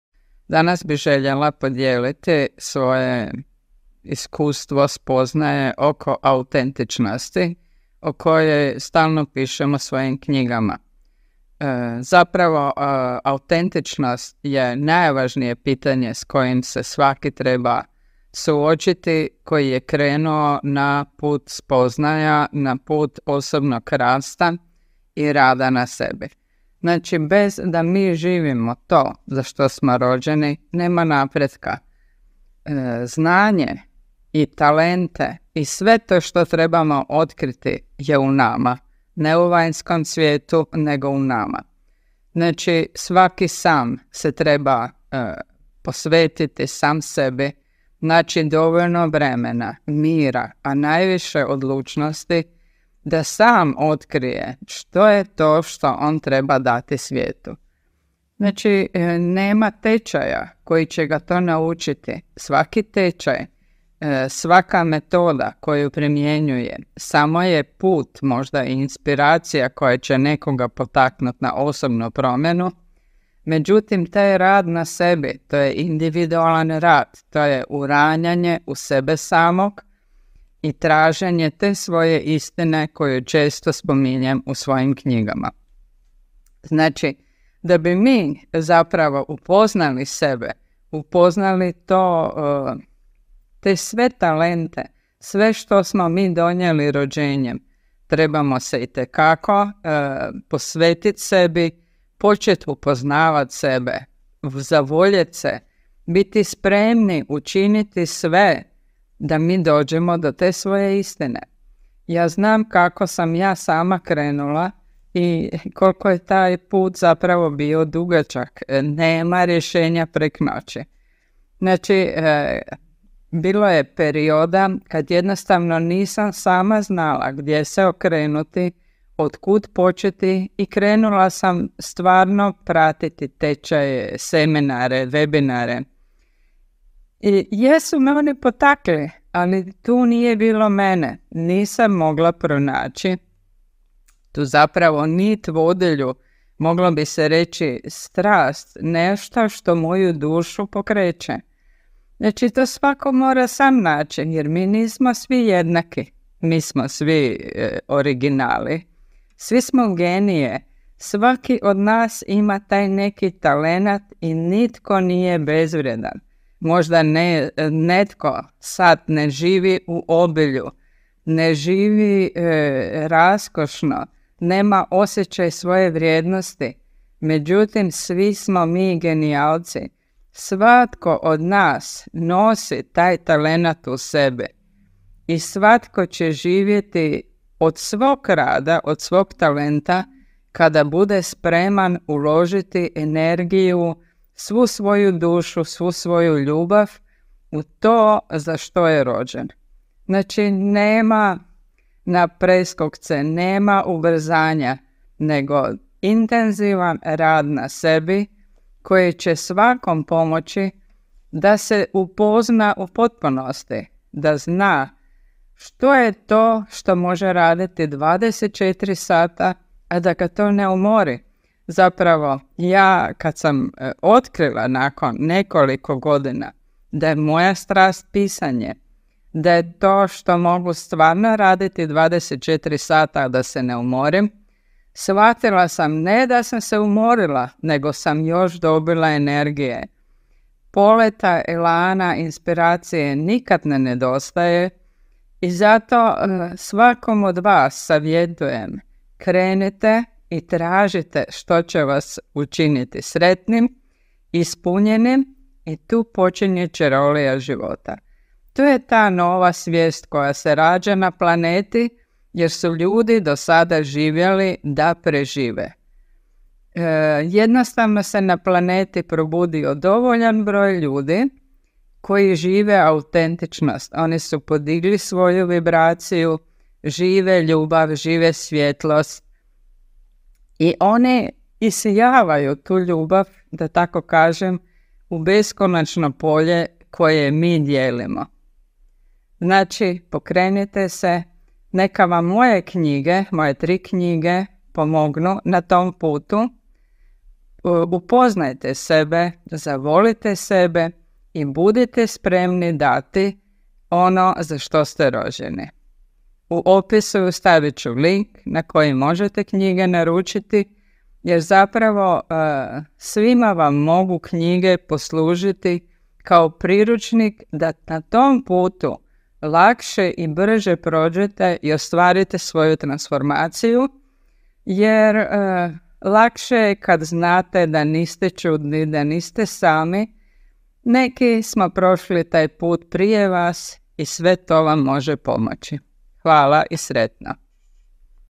Autentičnost nam otvara vrata Zlatnog doba
Kada živimo srcem mi dozvoljavamo Božanskoj inteligenciji da se izražava kroz nas. Kreativni smo, živimo ljubav i svjetlost. Osjećaj ispunjenosti i unutarnjeg mira preplavljuje cijelo naše biće.